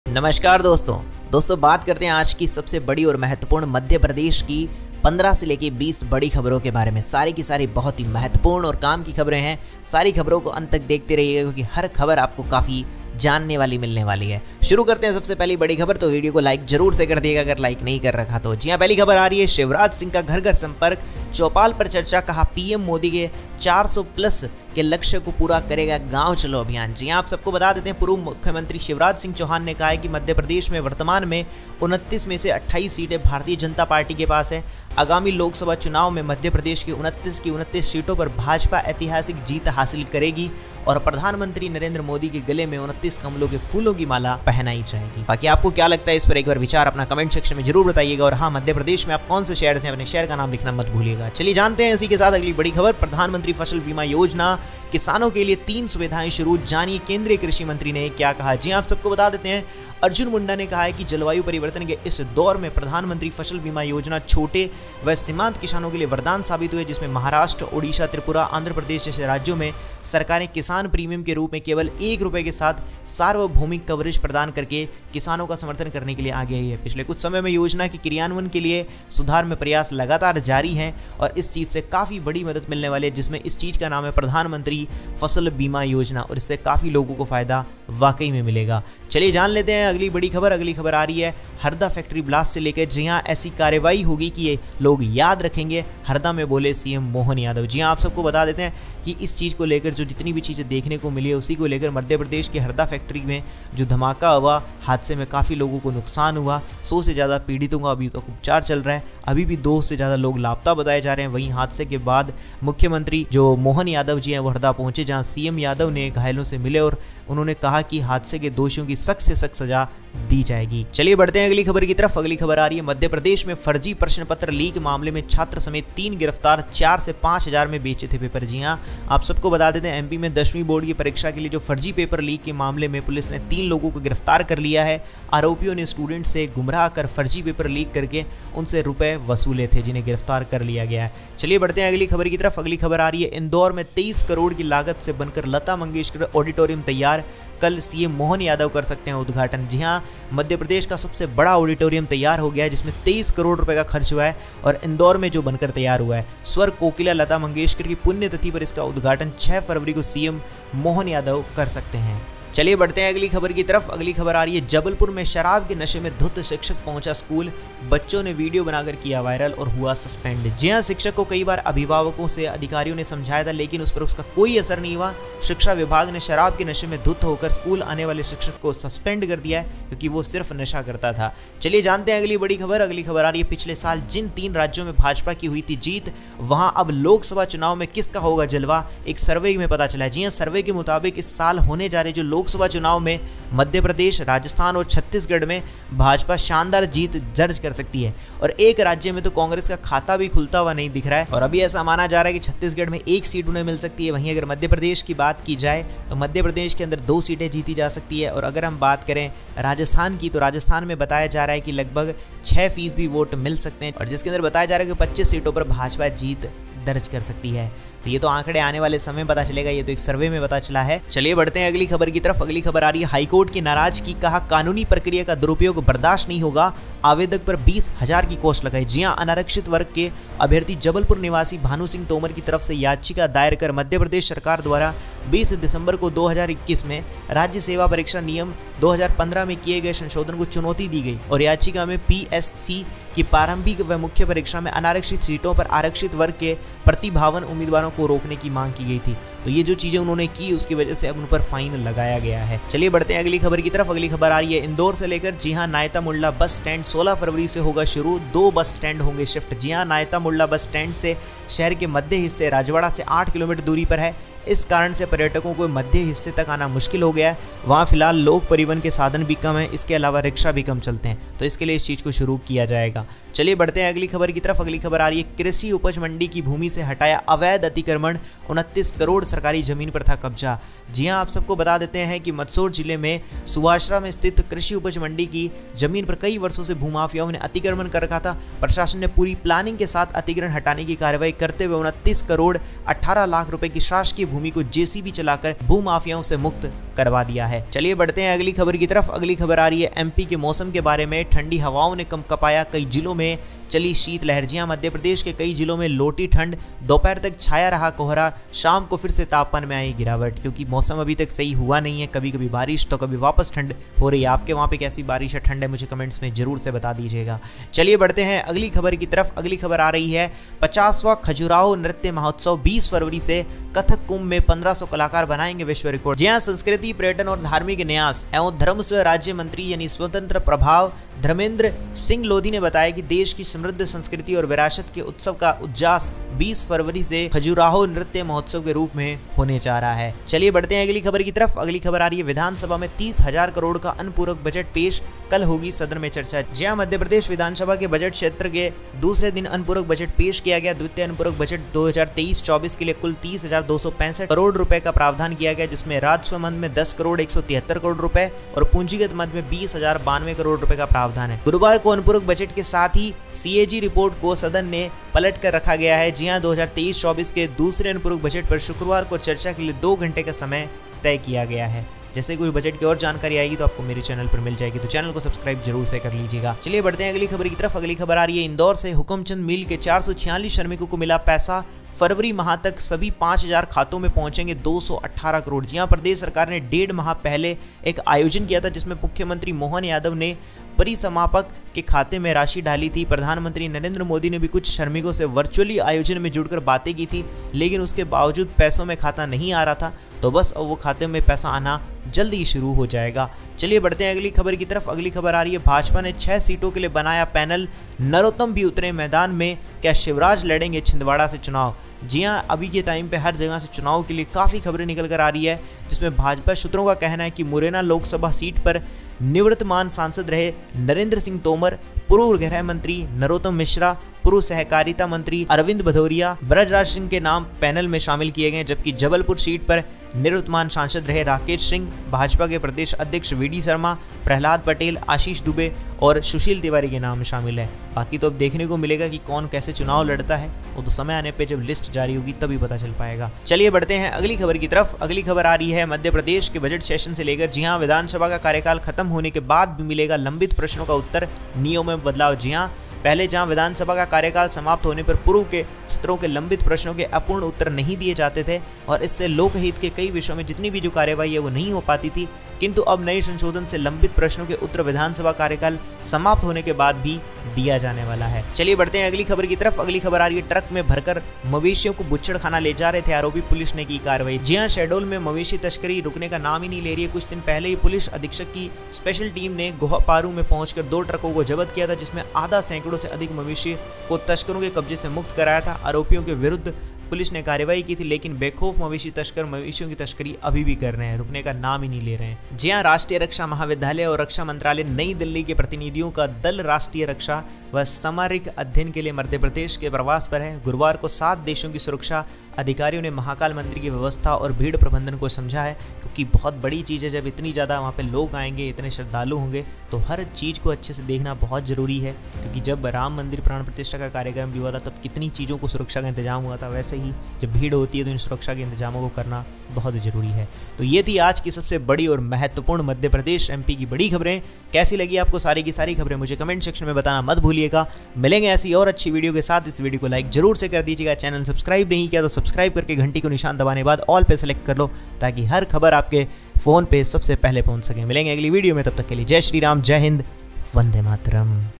14 February 2024 Madhya Pradesh Today Breaking News |MP CM Mohan|MP Board Exam,Ujjain,Budget,Harda F
9 February 2024 Madhya Pradesh Today Breaking News |MP CM Mohan|MP Board Exam,Ujjain,Budget,Harda Factory

शिवराज सिंह का घर-घर संपर्क, चौपाल पर चर्चा, कहा-PM मोदी के 400+के लक्ष्य को पूरा करेगा गांव चलो अभियान

प्रधानमंत्री फसल बीमा योजना: किसानों के लिए 3 सुविधाएं शुरु, जानिए केंद्रीय कृषि मंत्री ने क्या कहा?

Harda Factory Blast: 'ऐसी कार्रवाई होगी कि ये लोग याद रखेंगे...', हरदा में बोले CM मोहन यादव

MP Board Exam: मध्य प्रदेश में 'फर्जी' प्रश्न पत्र लीक मामले में छात्र समेत 3 गिरफ्तार, चार से पांच हजार में बेचे थे पेपर

MP News: इंदौर में 23 करोड़ की लागत से बनकर लता मंगेशकर ऑडिटोरियम तैयार, कल CM मोहन कर सकते हैं उद्घाटन

WATCH: जबलपुर में शराब के नशे में धुत शिक्षक पहुंचा स्कूल, बच्चों ने वीडियो बनाकर किया वायरल, सस्पेंड

पिछले साल जिन तीन राज्यों में भाजपा की हुई थी जीत, वहां अब लोकसभा चुनाव में किसका जलवा; आ गया सर्वे

Jabalpur: हाईकोर्ट की नाराजगी, कहा- कानूनी प्रक्रिया का दुरुपयोग बर्दाश्त नहीं, आवेदक पर 20 हजार की कॉस्ट लगाई

Indore News: नायता मुंडला बस स्टैंड 16 फरवरी से होगा शुरू, दो बस स्टैंड होगे शिफ्ट

Mandsaur: कृषि उपज मंडी की भूमि से हटाया अवैध अतिक्रमण, 29 करोड़ सरकारी जमीन पर था कब्जा

MP Weather Update: ठंडी हवाओं ने कंपकंपाया, कई जिलों में चली शीत लहर

MP News: 50वां खजुराहो नृत्य महोत्सव 20 फरवरी से, कथक कुंभ में 1500 कलाकार बनाएंगे विश्व रिकॉर्ड

MP News: विधानसभा में 30 हजार करोड़ का अनुपूरक बजट पेश, कल होगी सदन में चर्चा

Indore: हुकमचंद मिल के 446 श्रमिकों को मिला पैसा, फरवरी माह तक सभी पांच हजार खातों में पहुंचेगे 218 करोड़

MP: भाजपा ने छह सीटों के लिए बनाया पैनल, नरोत्तम भी उतरेंगे मैदान में, क्या शिवराज लड़ेंगे छिंदवाड़ा से चुनाव?

MP Budget Session: विधानसभा का कार्यकाल खत्म होने के बाद भी मिलेगा लंबित प्रश्नों का उत्तर, नियमों में बदलाव

Shahdol News: ट्रक में भरकर मवेशियों को बूचड़खाना ले जा रहे थे आरोपी, पुलिस ने की कार्रवाई

Ujjain News: रुद्राक्ष की माला से सजे बाबा महाकाल, सात देशों के अधिकारियों ने समझी भीड़ नियंत्रण की व्यवस्था

Shivraj Singh's door-to-door contact, discussion on Chaupal, said- Gaon Chalo campaign will fulfill PM Modi's target of 400+

Pradhan Mantri Fasal Bima Yojana: 3 facilities started for farmers, know what the Union Agriculture Minister said?

Harda Factory Blast: 'Such action will be taken that these people will remember...', CM Mohan Yadav said in Harda

MP Board Exam: 3 arrested including student in case of 'fake' question paper leak in Madhya Pradesh, papers were sold for four to five thousand

MP News: Lata Mangeshkar Auditorium is ready in Indore at a cost of Rs 23 crore, CM Mohan can inaugurate tomorrow.

WATCH: Drunk teacher reaches school in Jabalpur, children make video viral, suspended

Who will dominate the Lok Sabha elections in the three states where BJP had won last year? the survey has arrived

Jabalpur: High Court displeased, said- misuse of legal process will not be tolerated, imposed a cost of Rs 20 thousand on the applicant

Indore News: Naita Mundla bus stand will start from 16th February, two bus stands will be shifted.

Mandsaur: Illegal encroachment removed from agricultural produce market land, Rs 29 crore government land was encroached upon

MP Weather Update: Cold winds shook, cold wave in many districts

MP News: 50th Khajuraho Dance Festival from February 20, 1500 artists will create world record in Kathak Kumbh.

MP News: Supplementary budget of Rs 30 thousand crore presented in the Assembly, discussion will be held in the House tomorrow

Indore: 446 workers of Hukamchand Mill got money, Rs 218 crore will reach all five thousand accounts by the month of February.

MP: BJP made panel for six seats, Narottam will also contest, will Shivraj contest elections from Chhindwara?

MP Budget Session: Answers to pending questions will be available even after the end of the assembly term, changes in rules

Shahdol News: The accused were taking cattle to the slaughterhouse in a truck, police took action

Ujjain News: Baba Mahakal adorned with Rudraksha garland, officials of seven countries understood the crowd control system.
Aasha Karta Hu Aapko Video Pasand Aayegi